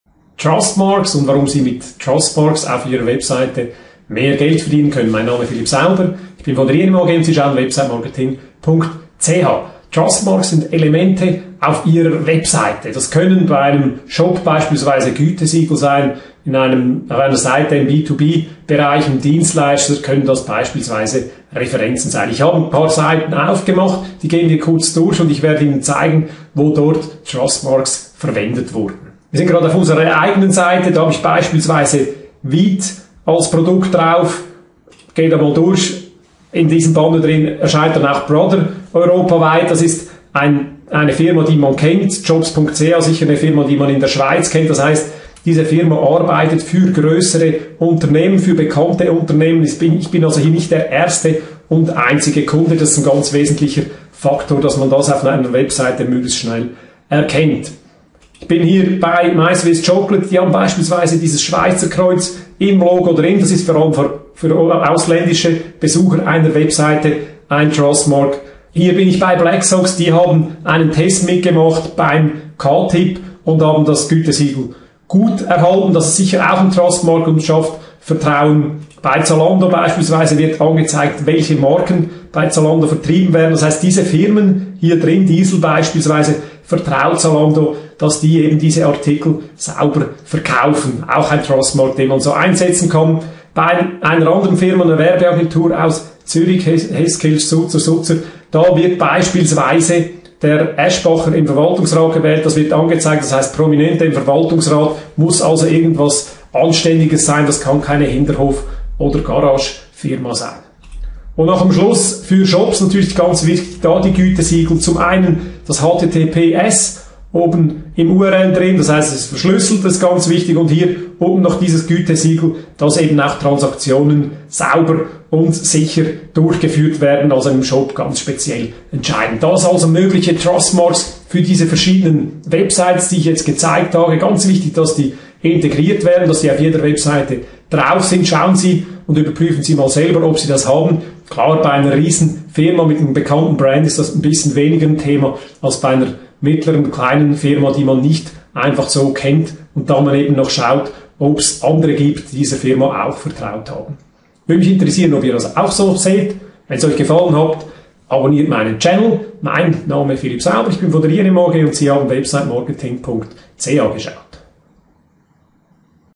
Trust Mark Marketing, ohne Vertrauen geht im Web gar nichts!
- Im Rahmen der Website Checks habe ich mich ums Thema Usability gekümmert und bin in dem Video der Frage nachgegaben: Was beziehungsweise welche Elemente auf einer Website schaffen Vertrauen. Im Marketing nennt man das Trust Marks, also Elemente die Vertrauen schaffen.
Im Video habe ich das an Hand einiger Sites analysiert und erläutert

Checken Sie Ihre Website!

Über Kommentare freue ich mich wie immer!

Philipp Sauber
INM AG